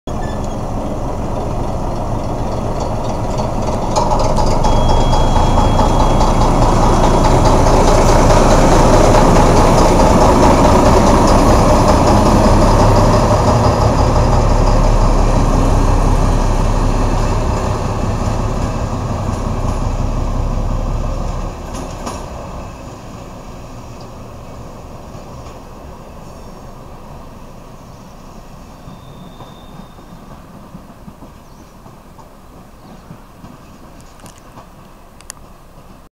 ČD 742.103 Veselí nad Moravou
Buchar 742.103 posunuje po stanici Veselí nad Moravou s přípojným vozem řady Bdtn, který byl odvěšen od vlaku Sp 1734.

Lokomotiva: CZ-ČD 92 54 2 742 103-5 (DKV Brno, PJ Veselí nad Moravou)
Datum: Čt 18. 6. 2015